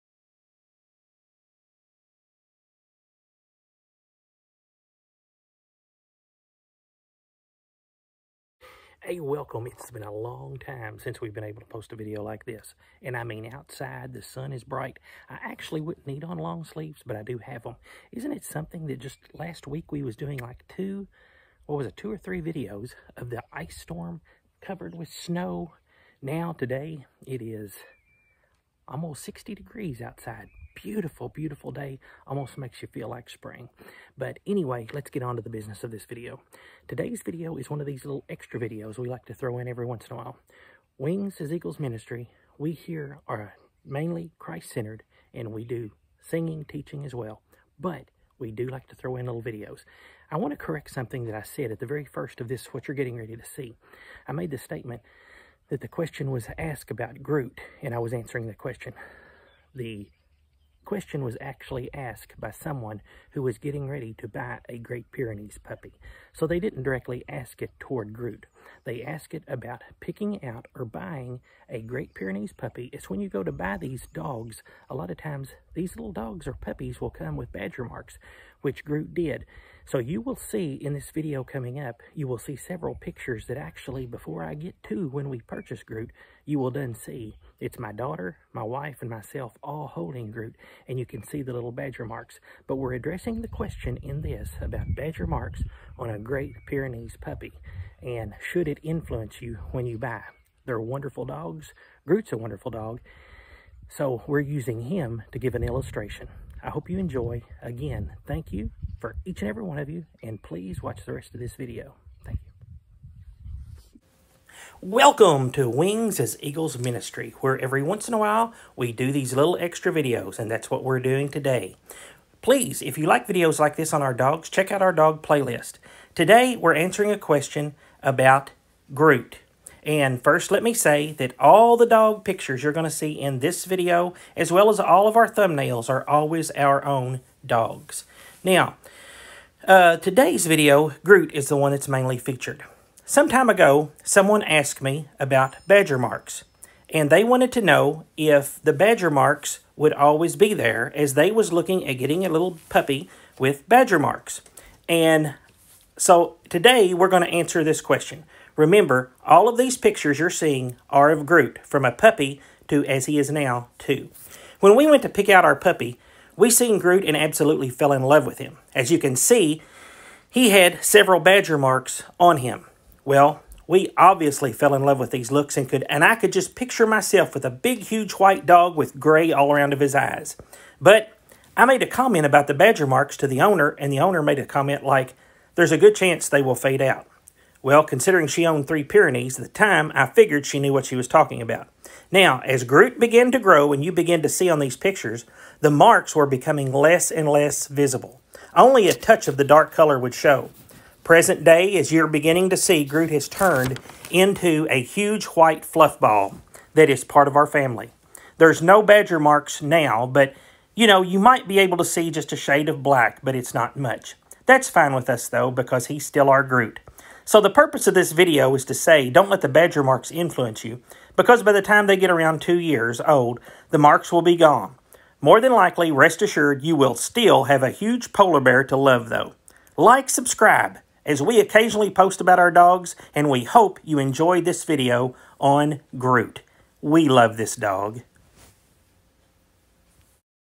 BUYING A GREAT PYRENEES PUPPY W/BADGER MARKS ..should this be deciding factor ? Starring Groot
Todays video  we discuss a question asked about GP puppies and badger marks….
This is a relatively short video but it shows quite a few pics of Groot as a puppy and how his looks have changed in two years.
Also in the pics are the rest of the WAEM , my wife and daughter.
I am NOT an expert on Great Pyrenees. I just own two and offer my advice occasionally.